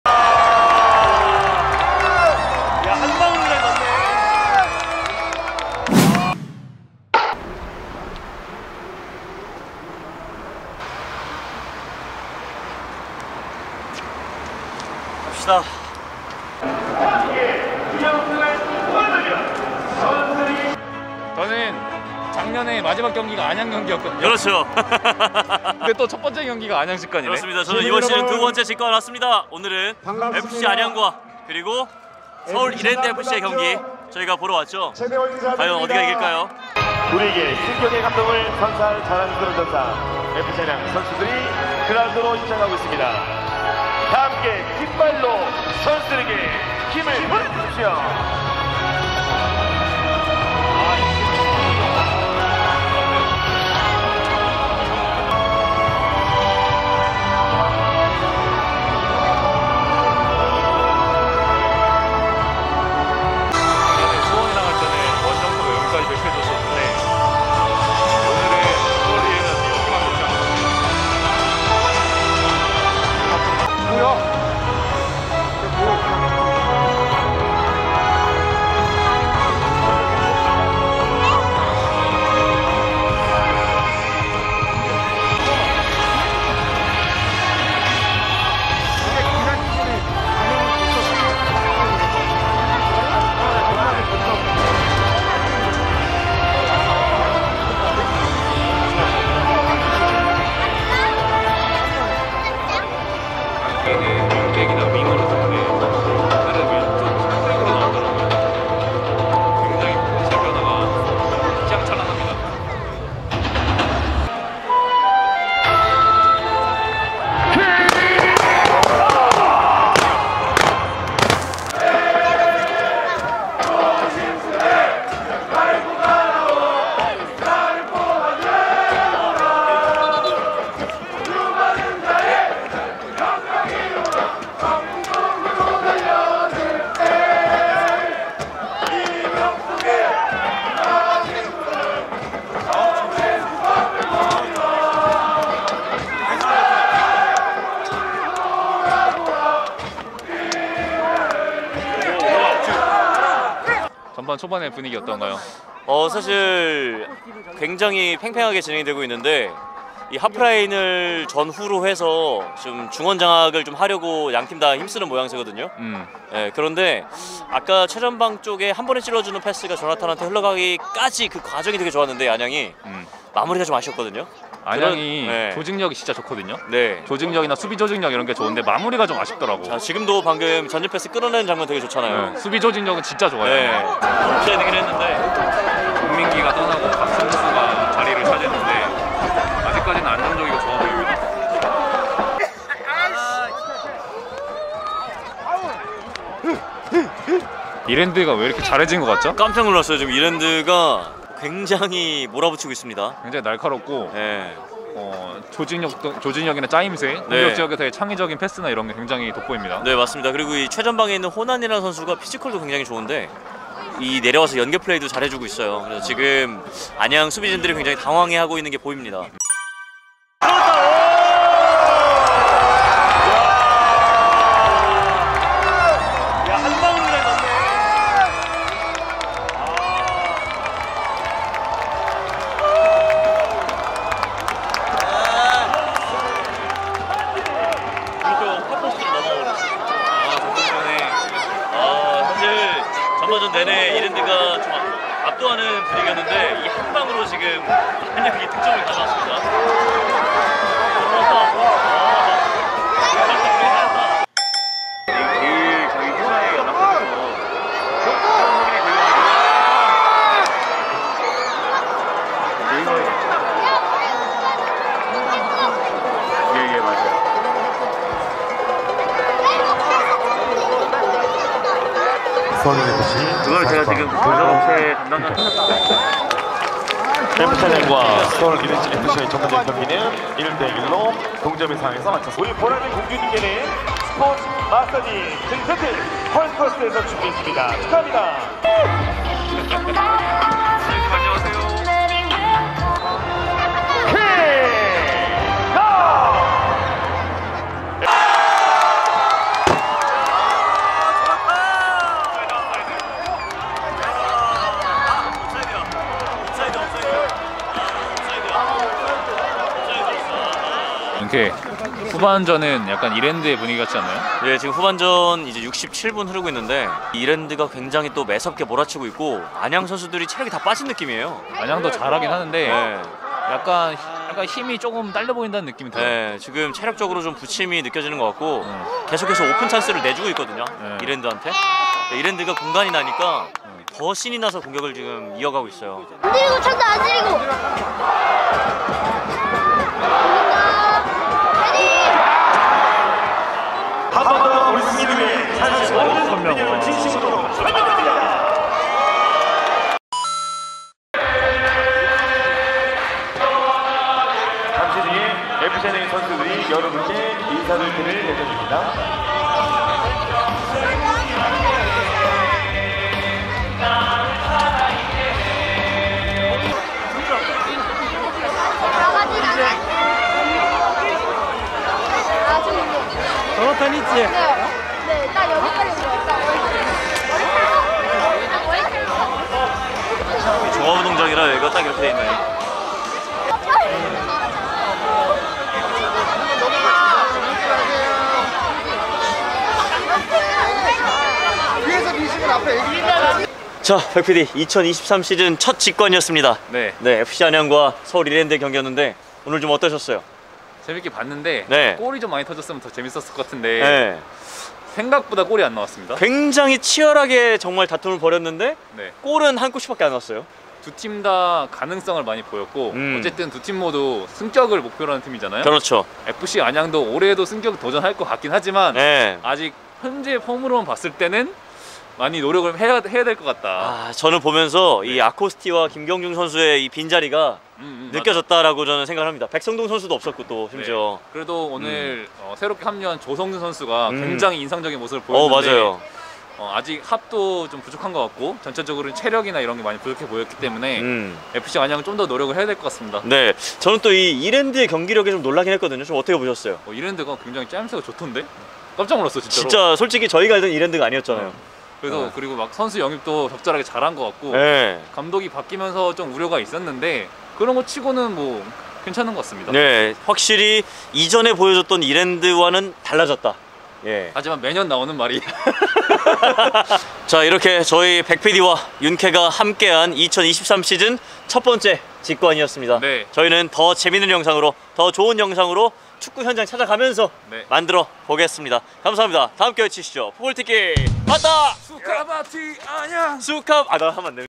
FC안양 올해는 K리그1 승격 가능?! ｜FC안양 vs 서울이랜드 직관 VLOG (ft. Nguyen Van Toan)
[풋볼티키빠따] 지난해 아쉽게 K리그1 승격에 실패한 FC안양이 다시 한번 승격을 노립니다. 2023년 10주년을 맞아 홈 개막전으로 서울 이랜드를 상대했는데요. 과연 승격을 노리는 두팀은 어떤 경기력을 보여줬을까요?!